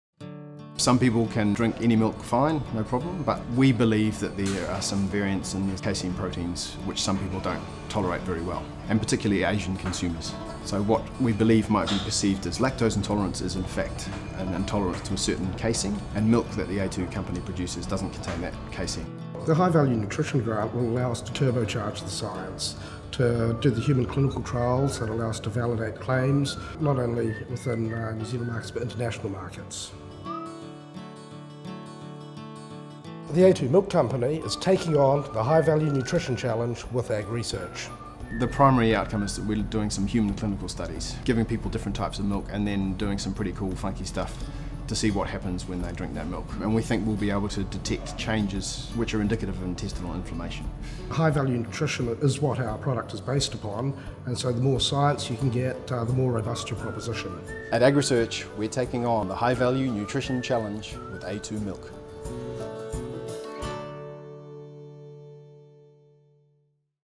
Aiding milk digestion – A new research project funded by High-Value Nutrition
Dr Matthew Barnett, Senior Research Scientist at AgResearch Ltd, and Dr Andrew Clarke, Chief Scientific Officer at a2 Milk, present a new project funded by the High-Value Nutrition National Science Challenge to research whether milk from a2 cows prevents intestinal inflammation, and are therefore suitable for people who don’t consume milk due to intolerance symptoms.